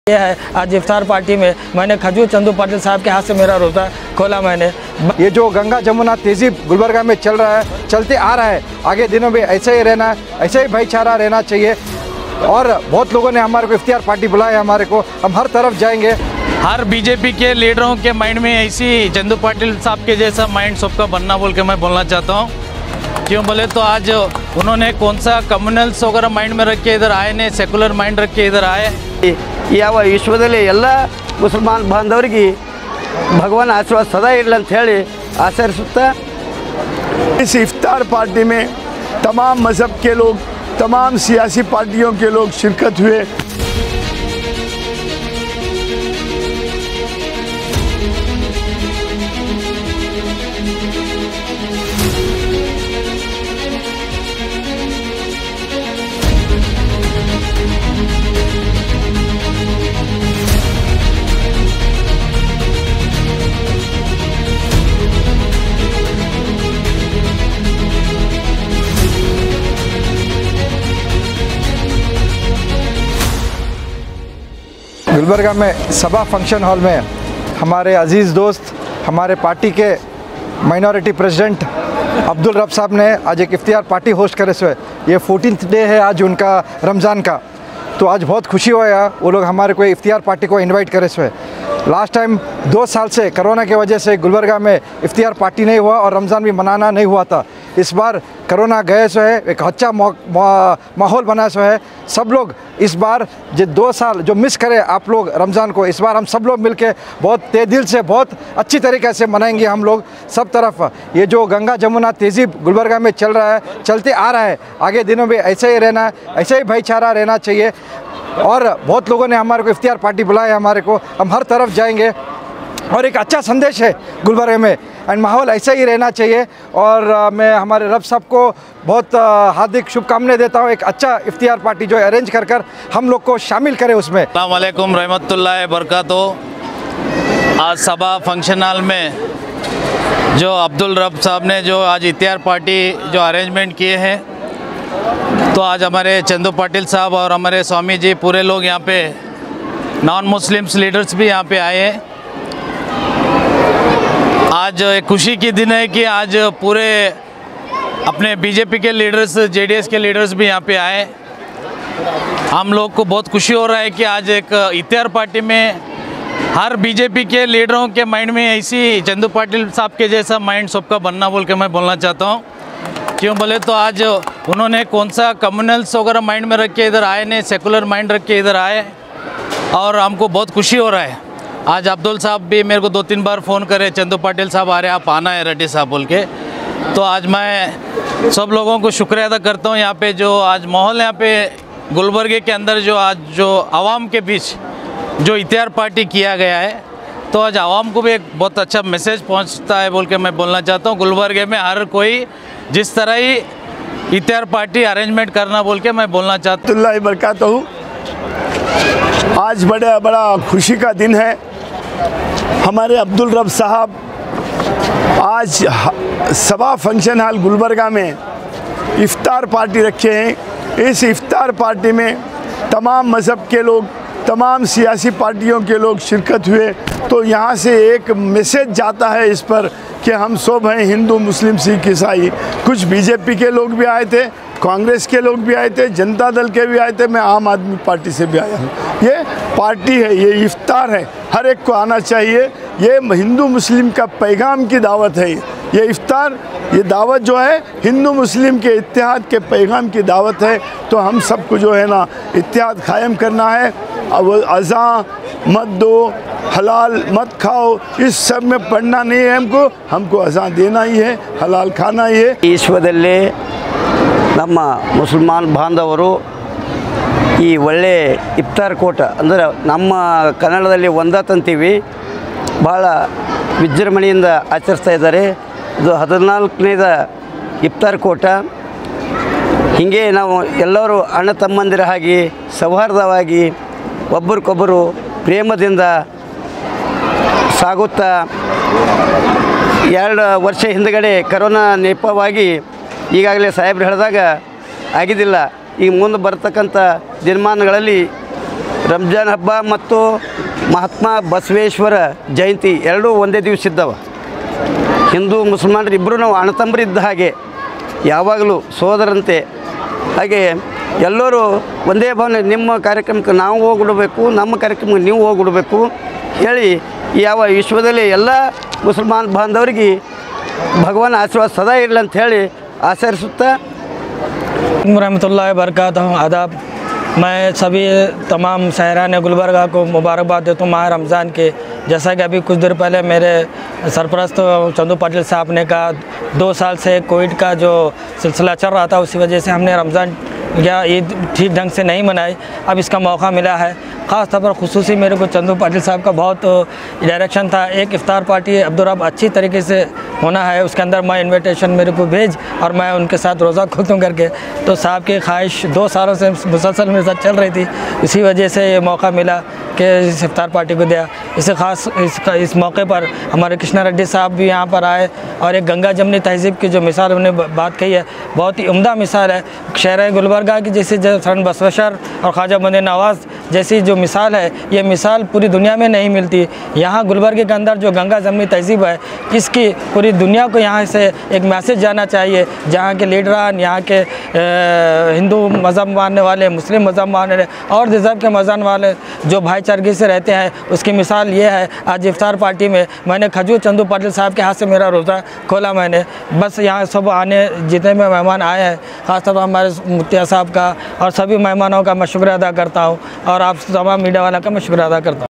Hindu Muslim Religious And Political Leaders Attend Iftar Party Organised By Abdul Rab BJP Leader
Hindu Muslim Religious And Political Leaders Attend Iftar Party Organised By Abdul Rab  BJP Dist Minority President Gulbarga